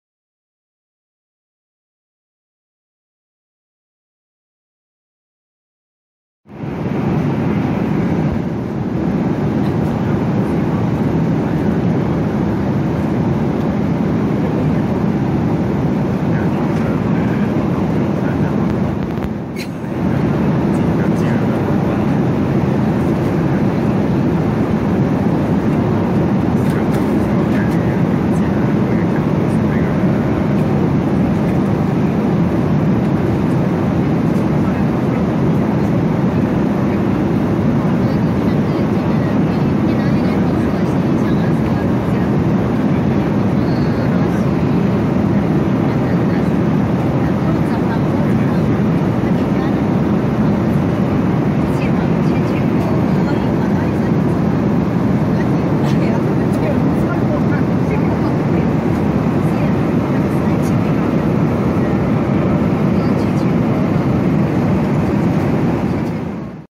國泰航空大阪關西國際機場往香港國際機場 (Studio CX自選音樂) - 2017年07月18日
錄影器材:Samsung Galaxy C7
剪輯軟件:Adobe Photoshop CS6,Corel VideoStudio X10,Microsoft PowerPoint 2016
影片解像度:1920x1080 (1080p,Full High Definition)
影片長度:1分0秒
航空公司:國泰航空 (Cathay Pacific)
飛機型號:Boeing 777-200 (雙發)
飛機航班:CX507 (大阪關西國際機場KIX往香港國際機場HKG)
起飛時間:17:53 (GMT+9日本時間)
降落時間:21:19 (GMT+8香港時間)
飛行距離:2978km
飛行時間:4時26分